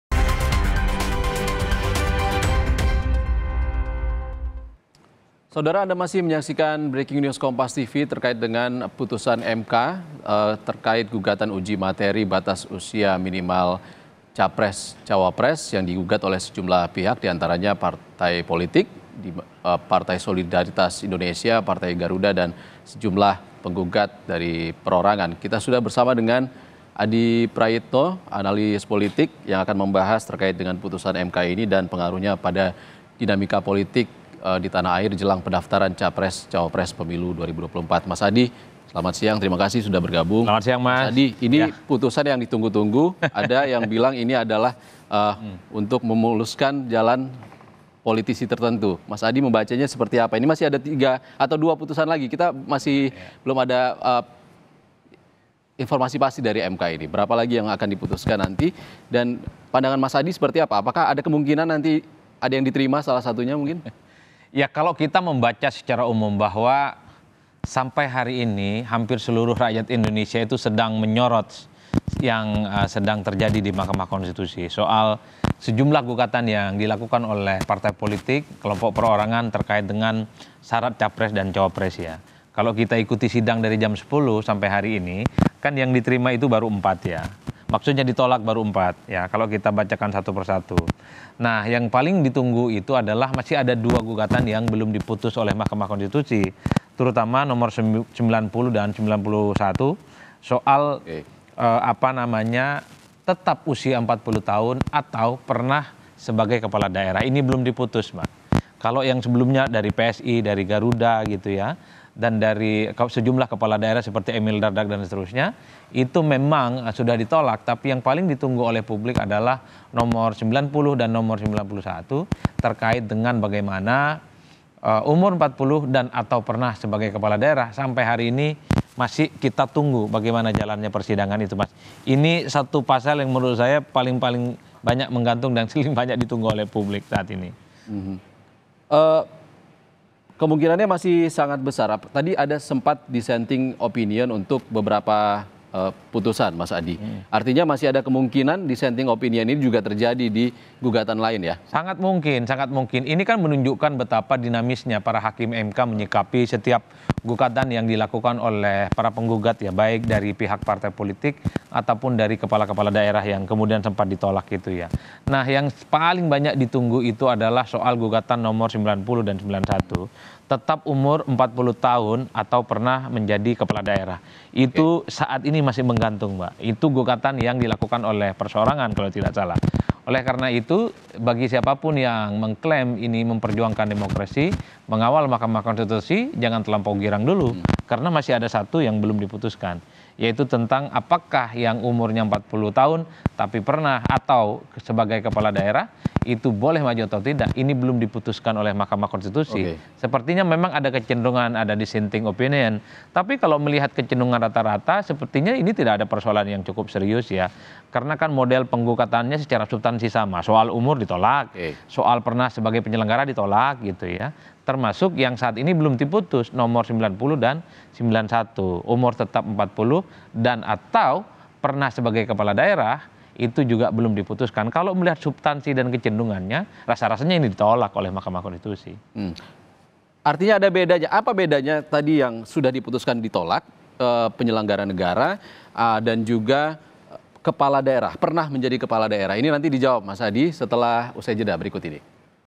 Analisis Pengamat Politik, Adi Prayitno soal Putusan MK yang Tolak Gugatan 35 Tahun Capres-Cawapres
KOMPAS. TV - Pengamat Politik, Adi Prayitno, mengangkat bicara terkait dengan Putusan Mahkamah Konstitusi (MK) yang menolak gugatan terkait batas usia 35 tahun untuk peserta Pemilihan Presiden (Pilpres), baik Calon Presiden maupun Wakil Presiden.

Menurut Adi Prayitno, alasan utama dari MK dalam menolak gugatan ini adalah bahwa usia minimal 40 tahun tetap dianggap sebagai syarat bagi Calon Presiden dan Wakil Presiden berdasarkan UUD 1945, dan hal ini merupakan kebijakan pembuat undang-undang yang sah.

Adi Prayitno juga mencatat bahwa MK menolak argumen PSI terkait Perdana Menteri Sjahrir yang berusia di bawah 40 tahun, yang menunjukkan bahwa MK memandang usia minimal 40 tahun sebagai syarat yang berlaku untuk Capres dan Cawapres.

Meskipun kontroversi seputar batas usia ini telah banyak diperbincangkan, MK memutuskan untuk mempertahankan aturan tersebut dan menganggapnya sebagai ranah kebijakan pembuat undang-undang.

Dengan demikian, putusan MK ini tidak dilihat sebagai bentuk diskriminasi usia, melainkan sebagai interpretasi hukum yang mengacu pada UUD 1945 dan memandang usia 40 tahun sebagai syarat yang berlaku untuk Capres dan Cawapres.